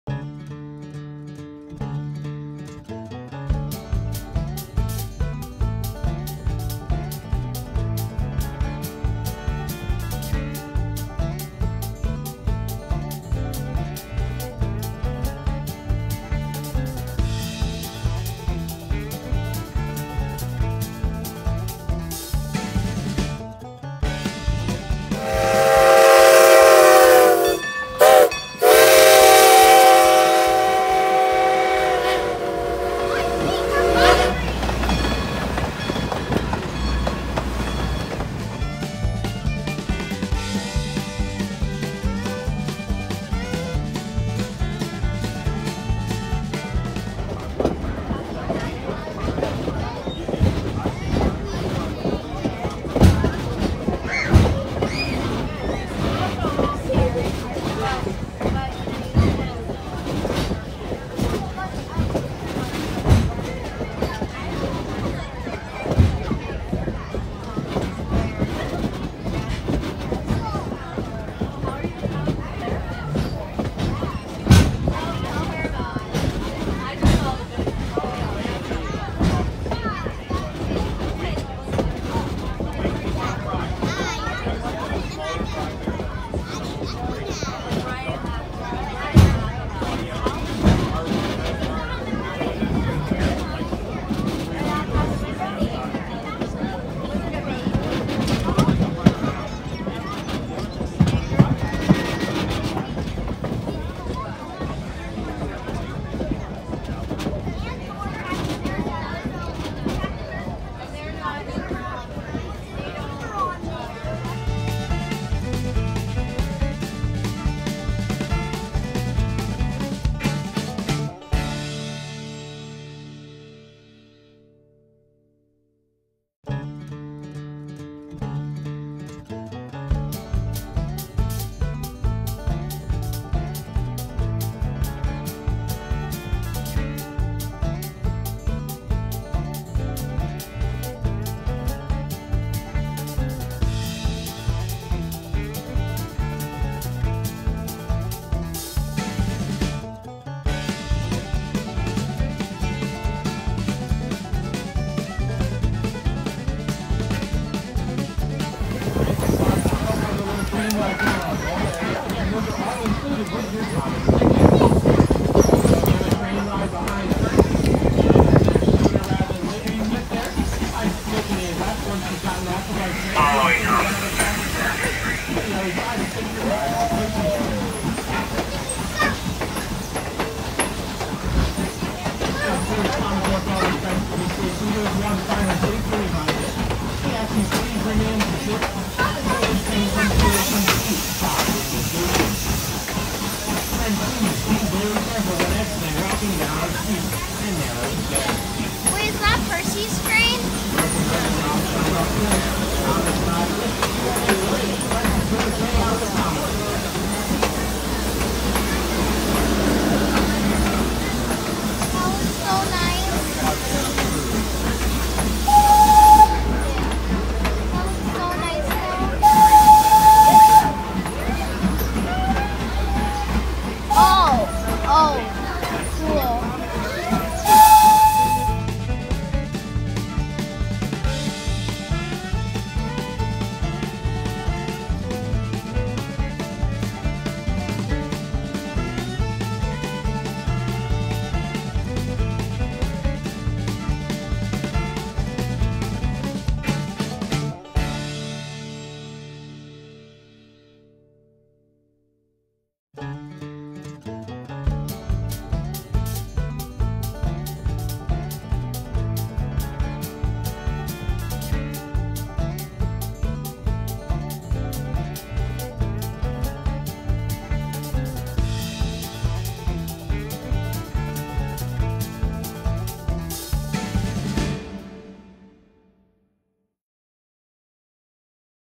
Strasburg PA - Trains - Toys - Thomas - The Red Caboose Motel
We made our last trip to The Red Caboose Motel around June of 2019, hopefully we will go again soon.
Here are some videos and pictures I took during that trip.
We stayed at the Red Caboose Motel. While there we went to the National Toy Train Museum, The Strasburg Railroad, and the Railroad Museum of PA.
I wish I had taken more pictures and videos; the toy train museum had a bunch of great layouts and displays, there were so many more trains to see, and the hands on G scale layout in the railroad museum is lots of fun. Guess I’ll just have to schedule another trip and get some better videos this time!